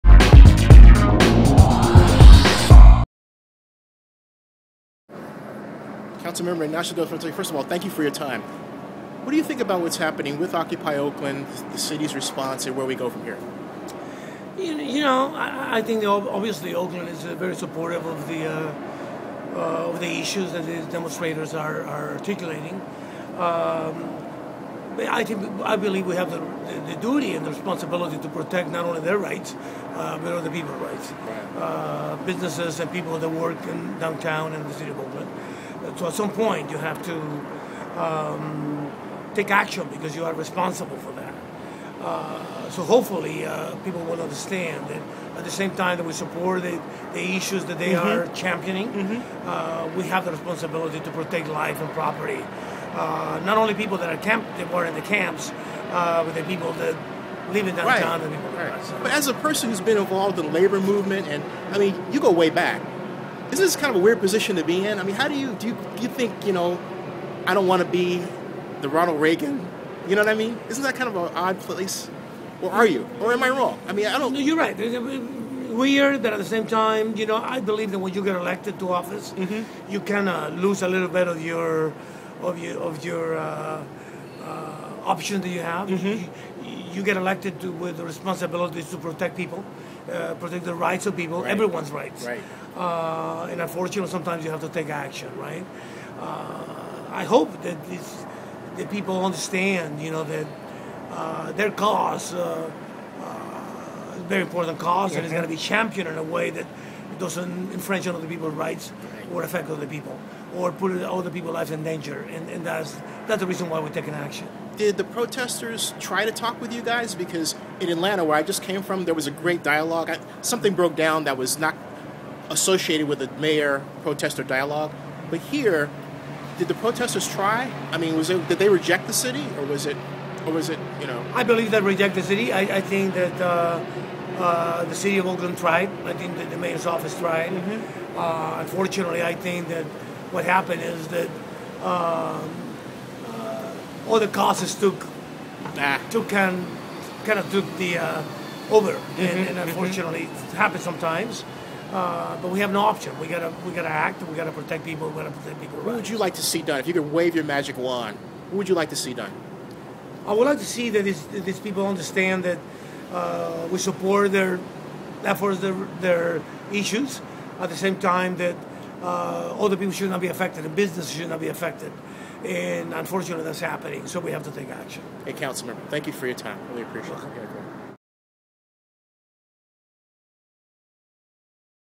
Ignacio De La Fuente : Oakland Councilmember On Occupy Oakland
Zennie62 Ignacio De La Fuente : Oakland Councilmember On Occupy Oakland. Ignacio was good enough to talk with me on the Tuesday October 25th night that would eventually lead to one of the most violent chapters in Oakland's history. - vlog produced by Zennie Abraham Zennie62 Blog - youtube video